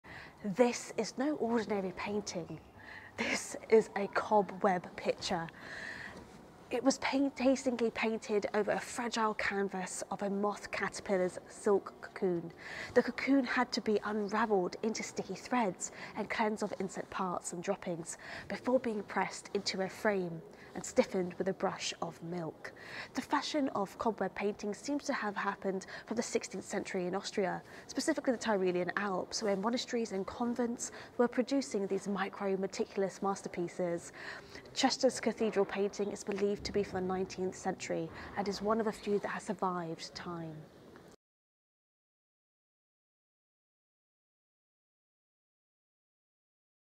Painting made from a Moth Caterpillar! Medieval Cobweb picture
Do you want to start creating content but do not know where to begin? Need audio and/or video production services?